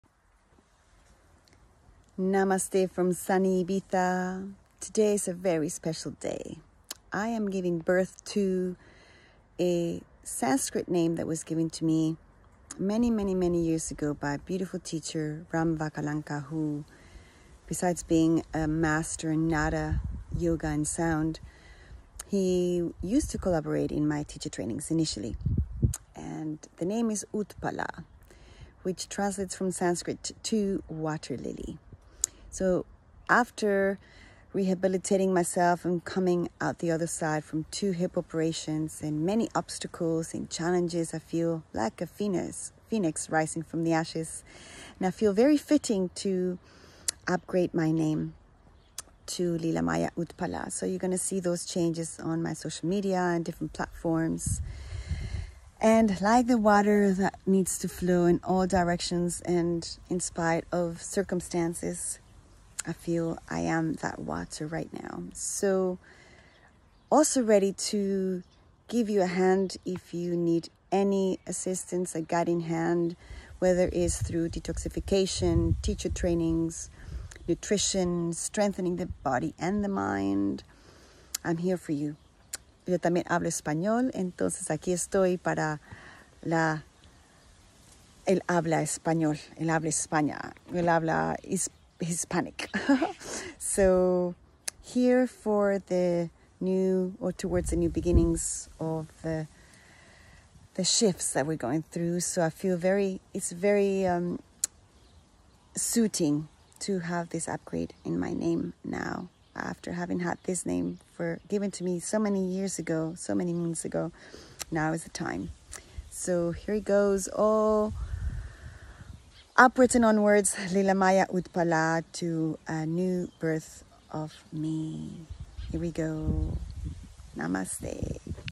Giving birth today today to my other spiritual name: 🪷UTPALA🪷
Utpala translates from Sanskrit to Water Lily, very fitting for our challenging times we are  going through as we ought to flow like water.